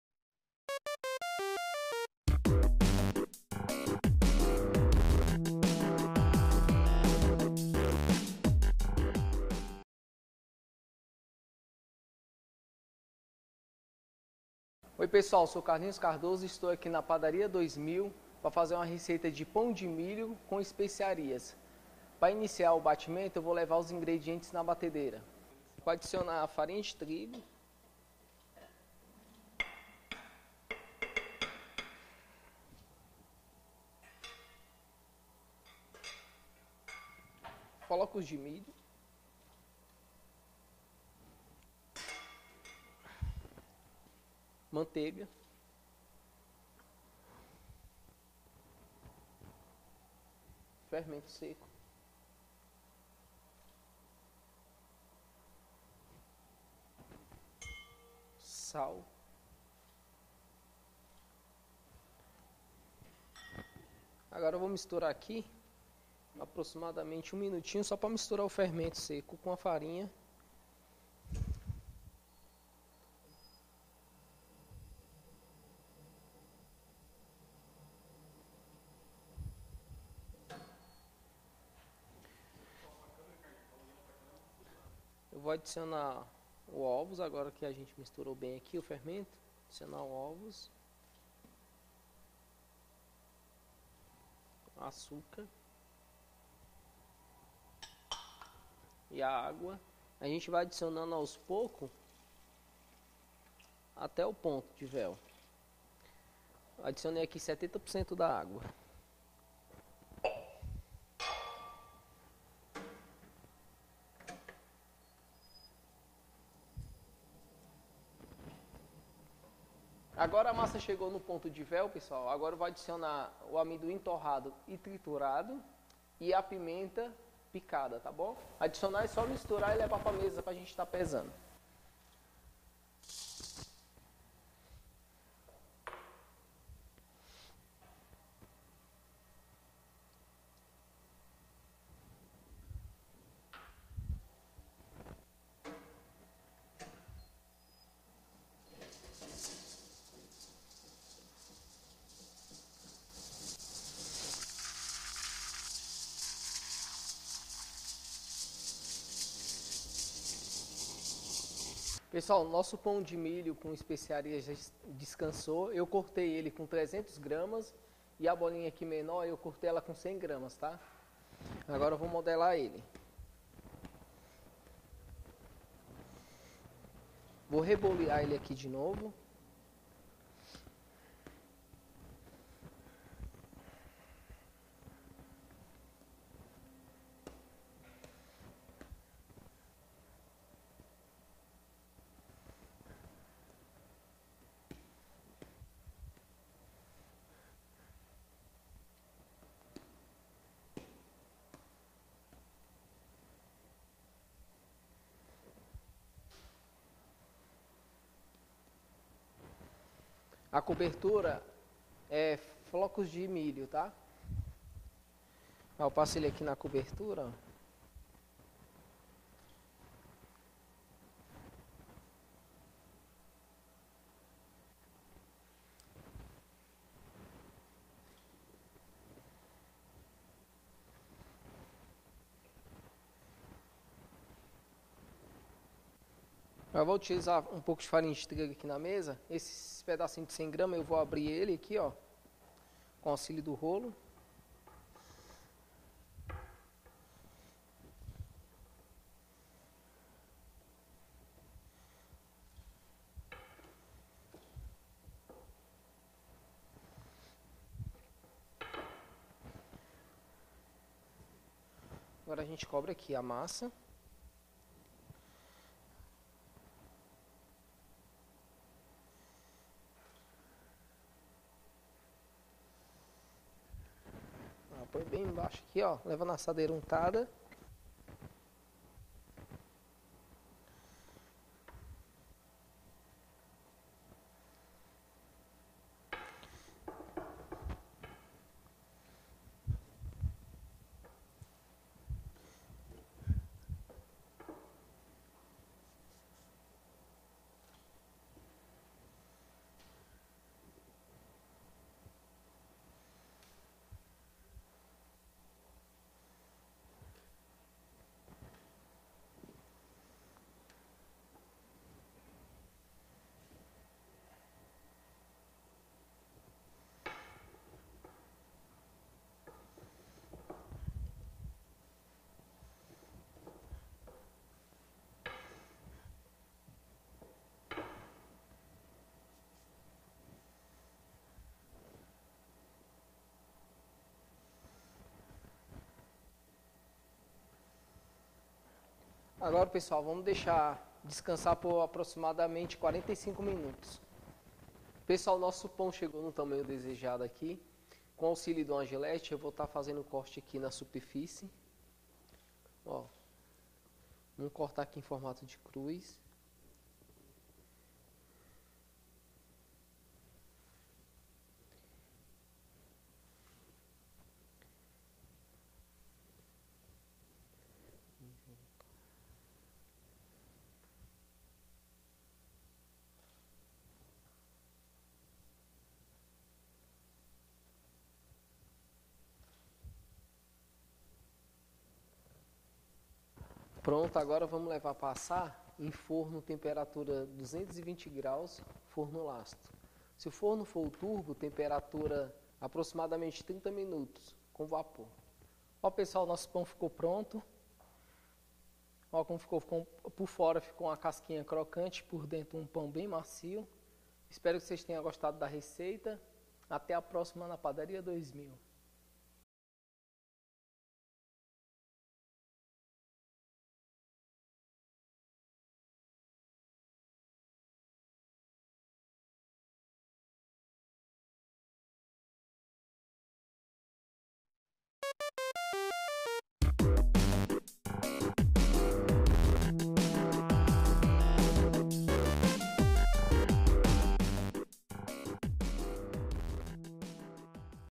Pão de Milho com Especiarias
Receita desenvolvida por Carlinhos Cardoso - Gran Chef Boulanger Cordão de Ouro 2014 São Paulo/SP

Ingredientes:
Farinha de trigo - 1.000g
Sal - 18g
Açúcar refinado - 55g
Farinha de milho - 400g
Ovos - 200g
Manteiga sem sal - 60g
Fermento seco - 20g
Água gelada - 350ml (aproximadamente)
Erva doce - 5g

Modo de Preparo:
1 - Coloque na masseira todos os ingredientes secos e misture bem. Depois, adicione o restante dos ingredientes e deixe bater até obter ponto de véu.
2 - Retire a massa da masseira, coloque na mesa, corte em pedaços desejados e deixe descansar por 30 minutos, coberto com um plástico.
3 - Modele os pães em seguida, passe sobre a farinha de milho, abra um pedaço de massa e cubra os pães. Arrume em uma assadeira lisa e leve para crescer em armário fechado por um tempo aproximado de 60 minutos.
4 - Faça cortes sobre os pães e leve para assar com as seguintes temperaturas: Forno tipo turbo a 140ºC. Forno tipo lastro a 180ºC, por um tempo aproximado de 25 minutos.